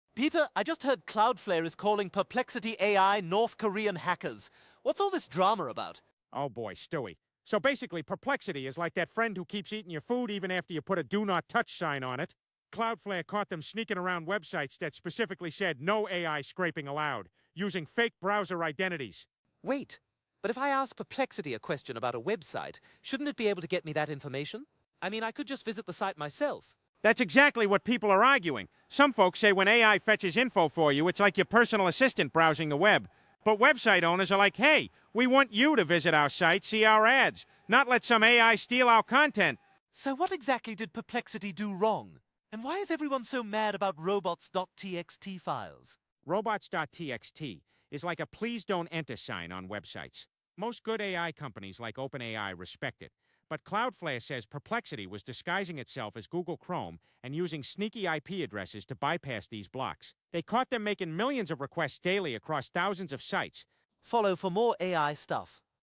AI vs. Web Scraping: When Does Help Become Harm?
Cloudflare just called out Perplexity AI for sneaky web scraping tactics - but some people are defending them! Is this AI helping users or harming the internet? We break down the controversy that's dividing tech Twitter.